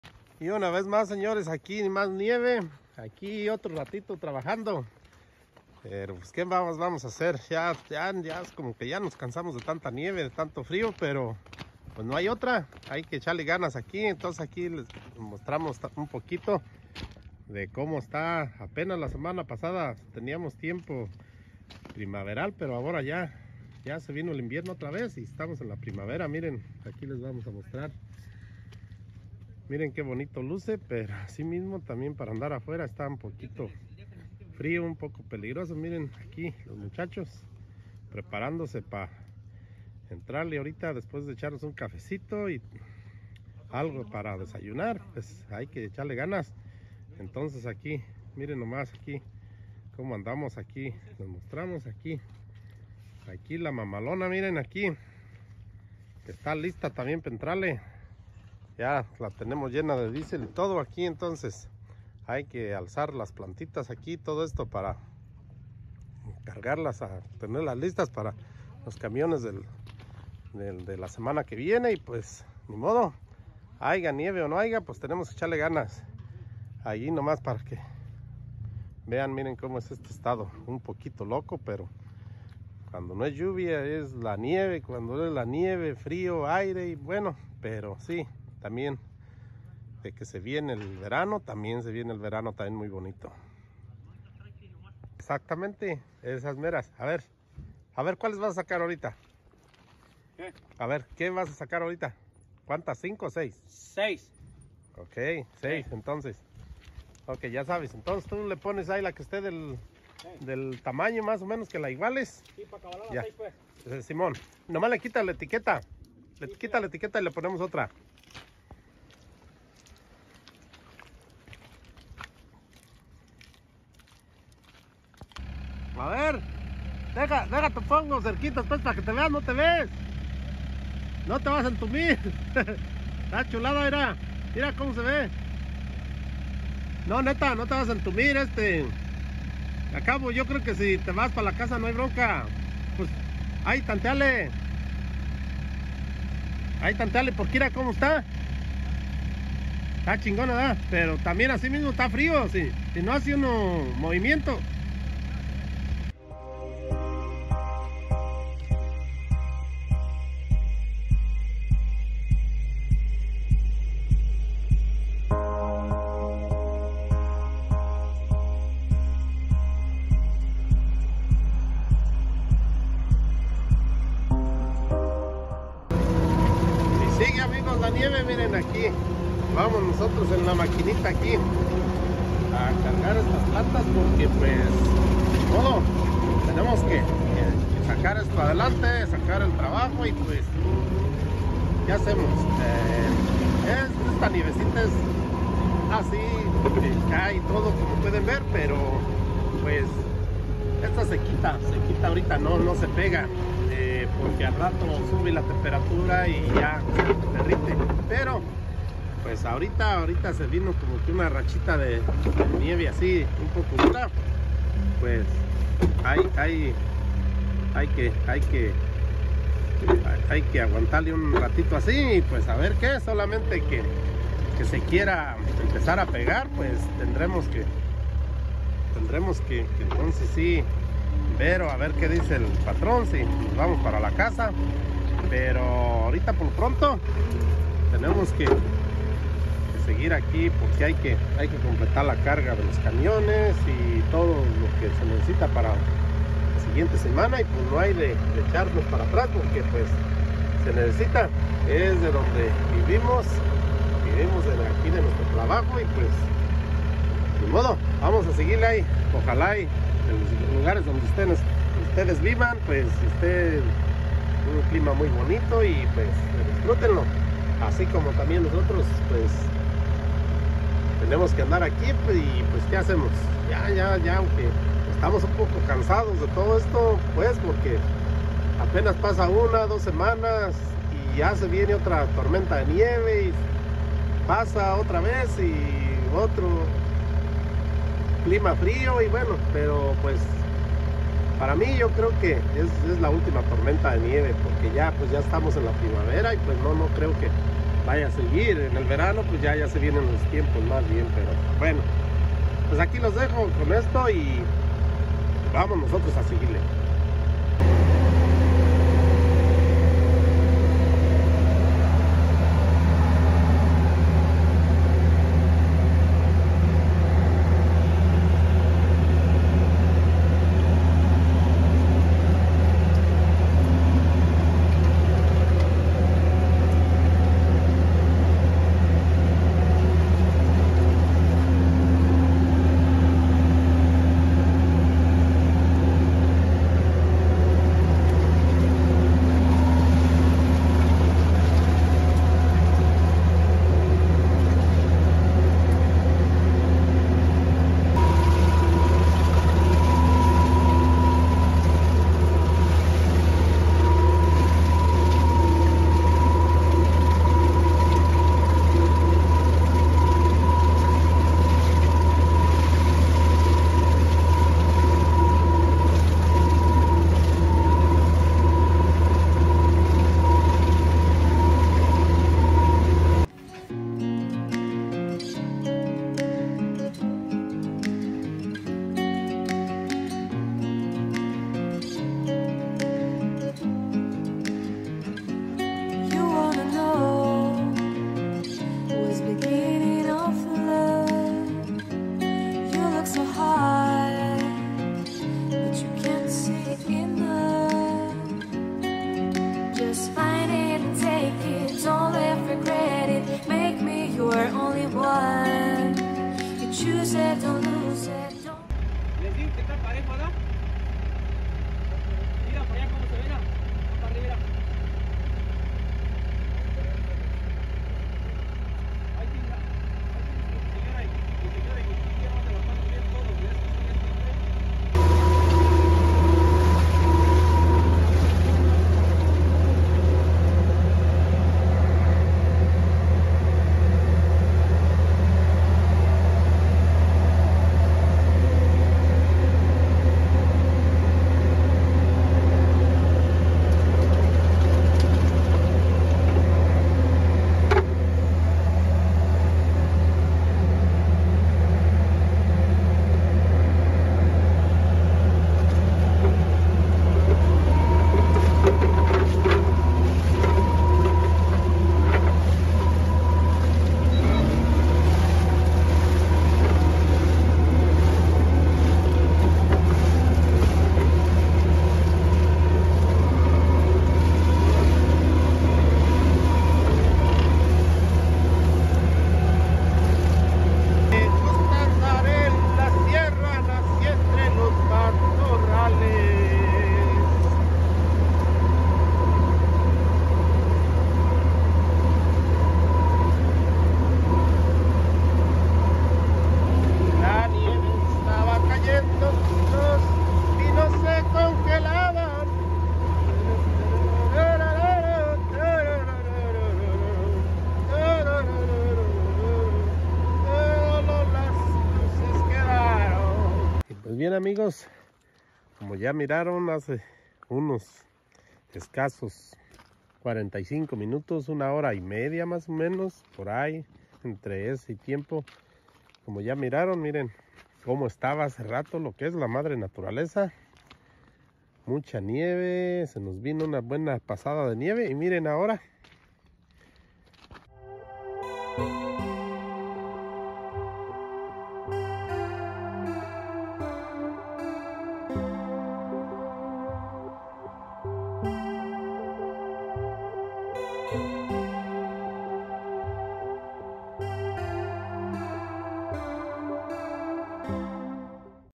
Otra vez nieve!! Ya estamos cansados de tanto frío
Ya estamos en primavera y otra vez nieve pero pues así es el clima en el estado de Oregón. Grasias. El vato michoacano